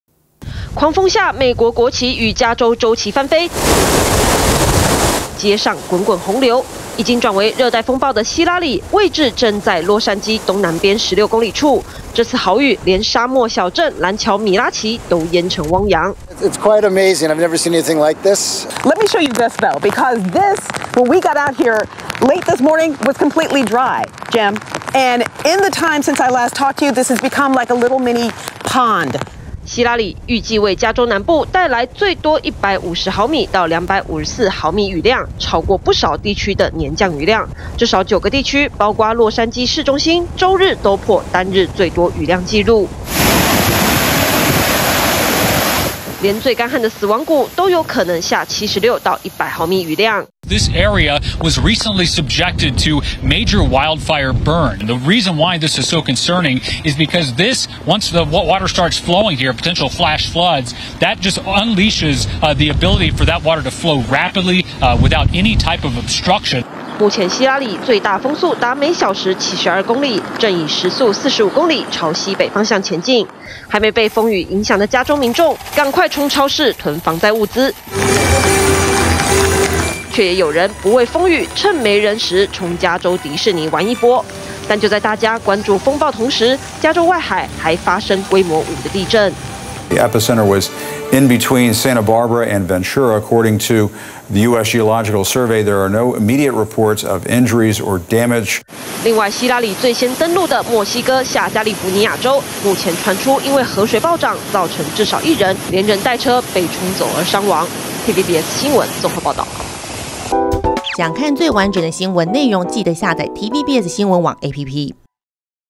沙漠小鎮也淹水! 「希拉里」襲加州 九地雨量破紀錄｜TVBS新聞
熱帶風暴【希拉里】，目前位置在洛杉磯東南方，這個風暴接近，讓加州降下驚人豪雨，至少有九個地方，打破單日最多雨量紀錄。連向來乾旱的加州死亡谷，都下100毫米的雨量。加州高速公路淹大水，多達12個郡，宣布進入緊急狀態。